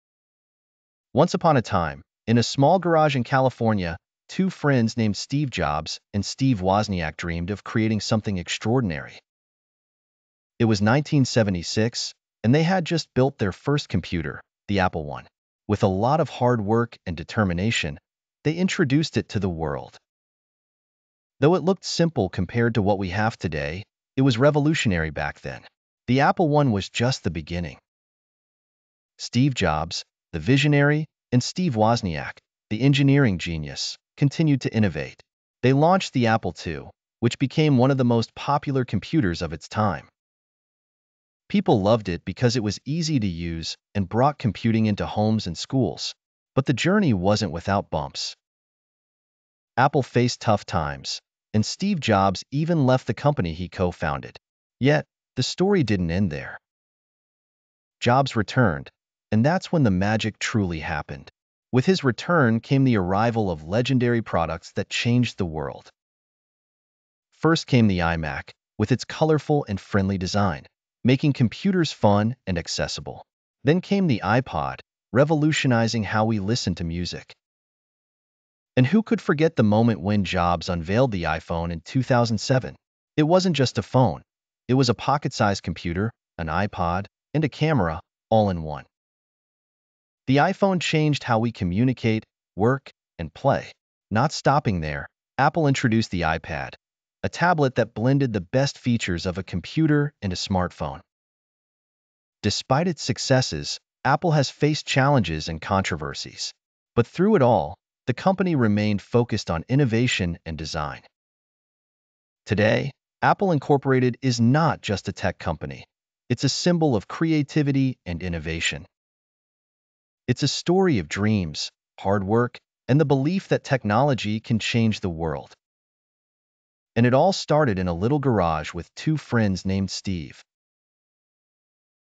The Apple Story From Garage to Global Innovation
From the launch of the Apple I by Steve Jobs and Steve Wozniak in 1976 to the revolutionary products like the iPhone and iPad, Apple Inc. has been a pioneer in technology, overcoming challenges and controversies to become a global leader in innovation and design.

❤️ روابط التواصل
✅ انستجرام